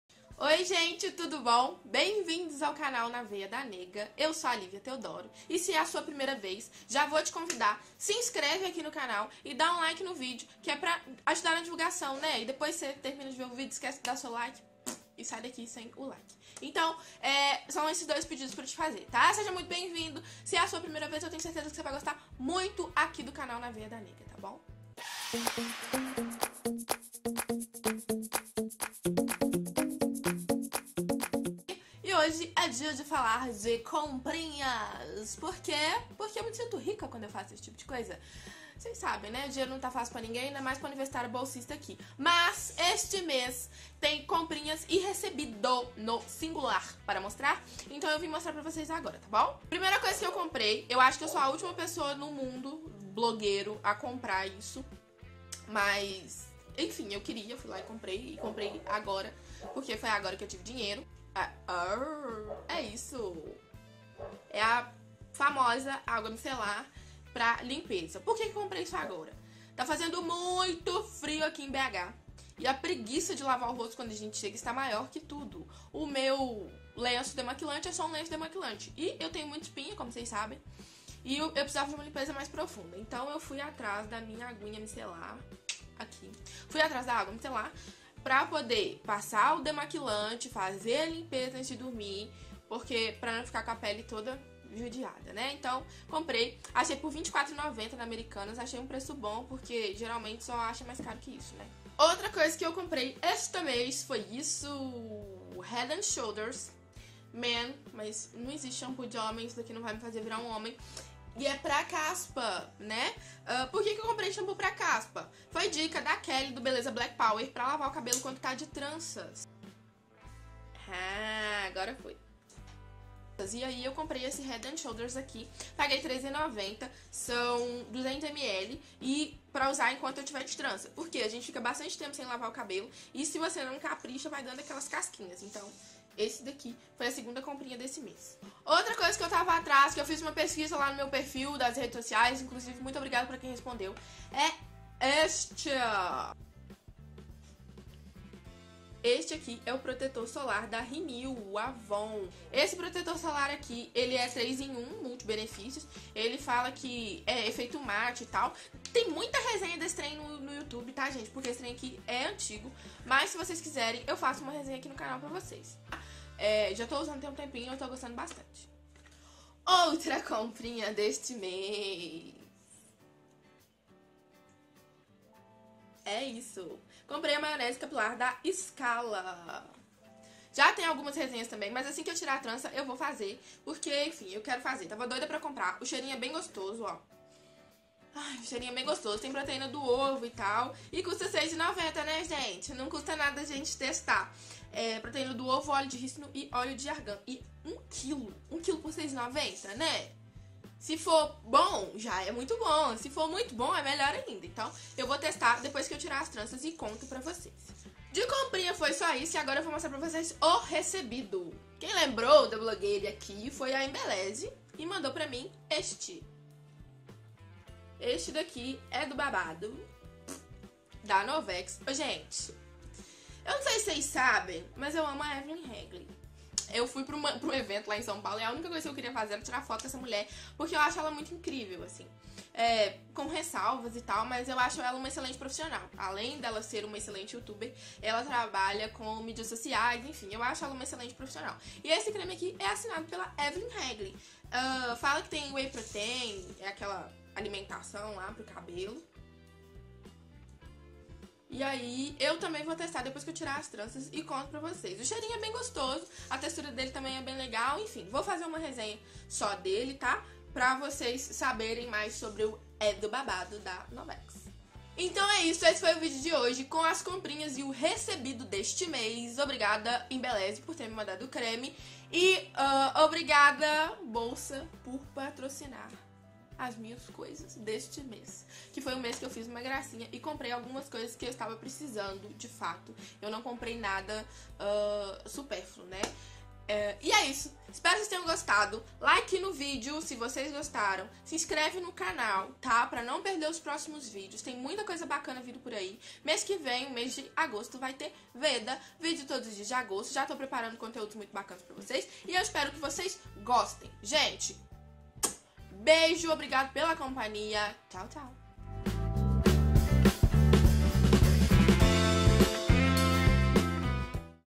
COMPRAS E RECEBIDOS DE JULHO | BELO HORIZONTE
Alguns produtinhos que comprei e um que recebi este mês. Se quiserem a resenha de algum deles, só deixar nos comentários!